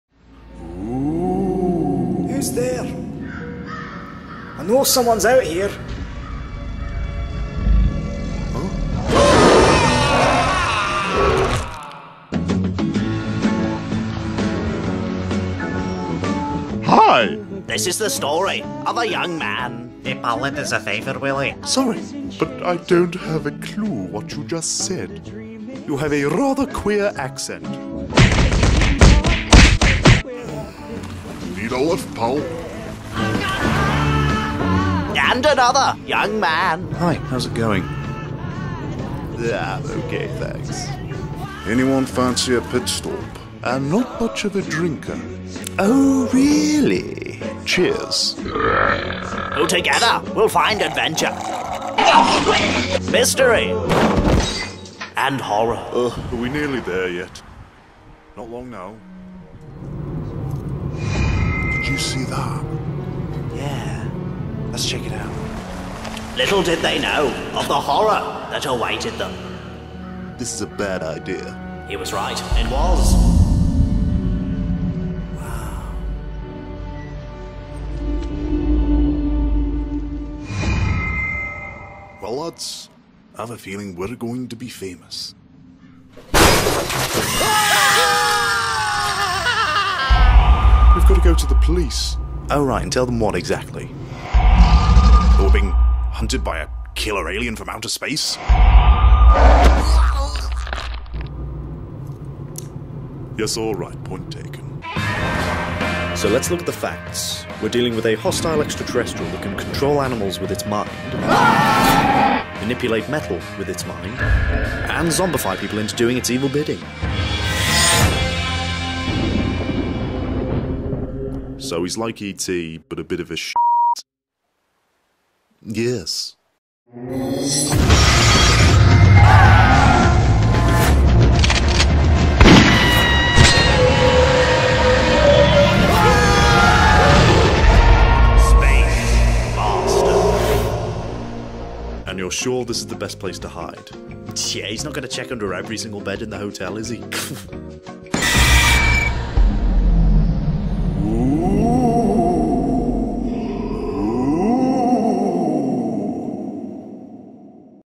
SPACE BASTARD
An old animatic trailer idea for a low budget comedy/horror/buddy film. (Done on Windows Paint, please forgive 😂)